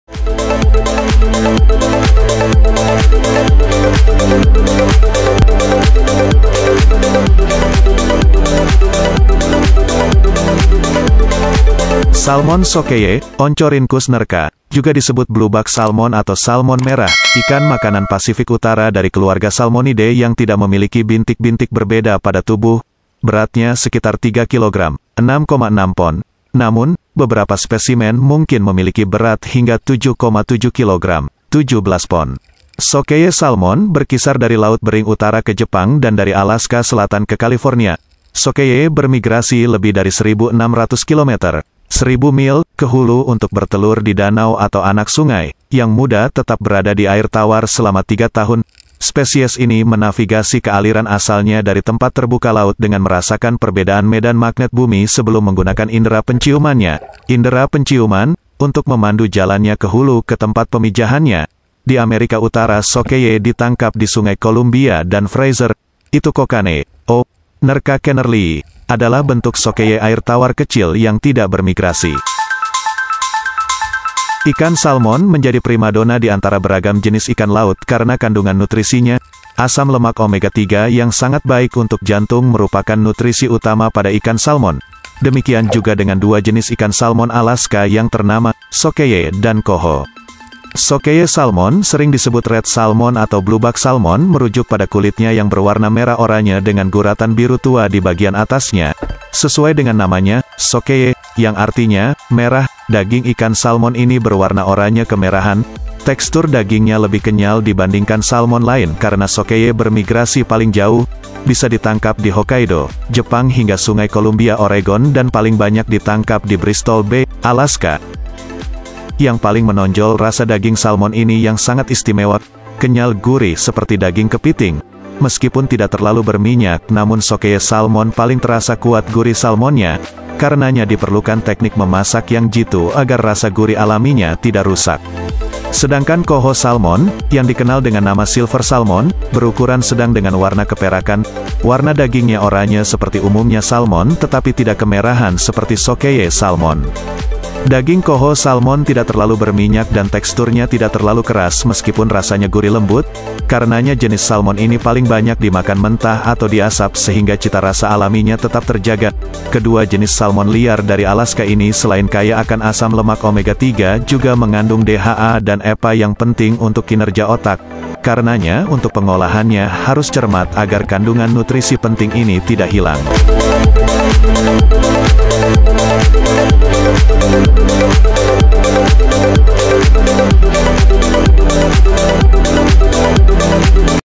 iKAN - ikan salmon sockeye (oncorhynchus nerka)
ikan sockeye salmon ( Oncorhynchus nerka ), juga disebut blueback salmon atau salmon merah , ikan makanan Pasifik Utara dari keluarga Salmonidae yang tidak memiliki bintik-bintik berbeda pada tubuh. Beratnya sekitar 3 kg (6,6 pon); namun, beberapa spesimen mungkin memiliki berat hingga 7,7 kg (17 pon). Sockeye salmon berkisar dari Laut Bering utara ke Jepang dan dari Alaska selatan ke California . Sockeye bermigrasi lebih dari 1.600 km (1.000 mil) ke hulu untuk bertelur di danau atau anak sungai, yang muda tetap berada di air tawar selama tiga tahun. Spesies ini menavigasi ke aliran asalnya dari tempat terbukalaut dengan merasakan perbedaan medan magnet bumi sebelum menggunakan indera penciumannya (indera penciuman ) untuk memandu jalannya ke hulu ke tempat pemijahannya. Di Amerika Utara sockeye ditangkap di sungai Columbia dan Fraser . Itukokanee  (nerka kennerlyi ) adalah bentuk sockeye air tawar kecil yang tidak bermigrasi.